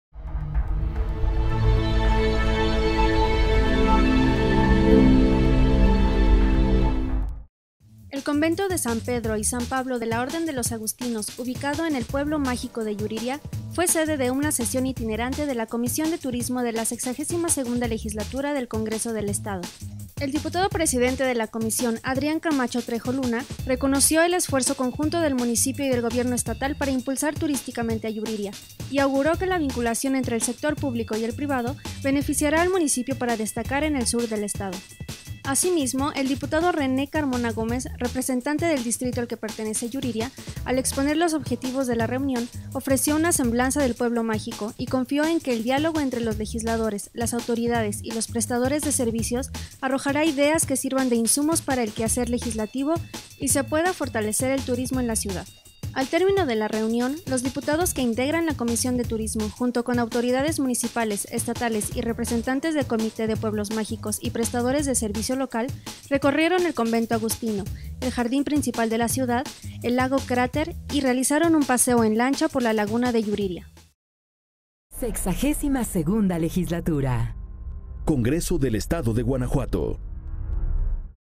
Comisión Itinerante de Turismo
El convento de San Pedro y San Pablo, de la orden de los Agustinos ubicado en el Pueblo Mágico de Yuriria, fue sede de una sesión itinerante de la Comisión de Turismo de la LXII Legislatura del Congreso del Estado.